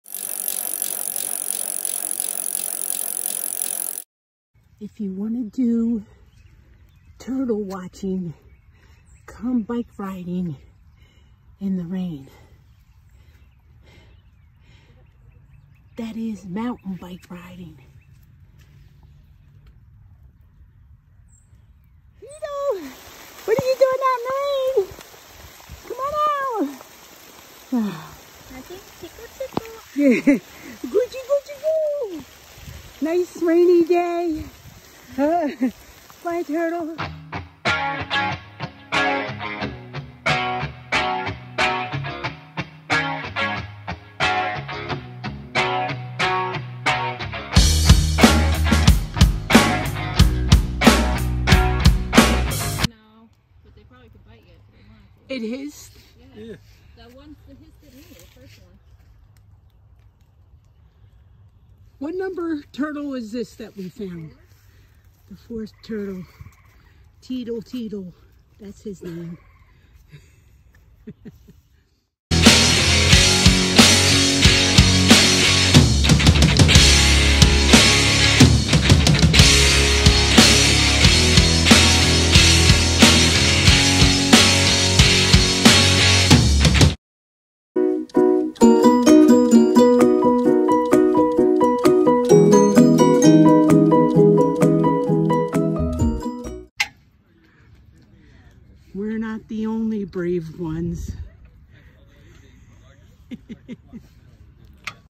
Mountain Biking with the Turtles
We got caught in a thunderstorm and ended up relocating turtles for the rest of the ride.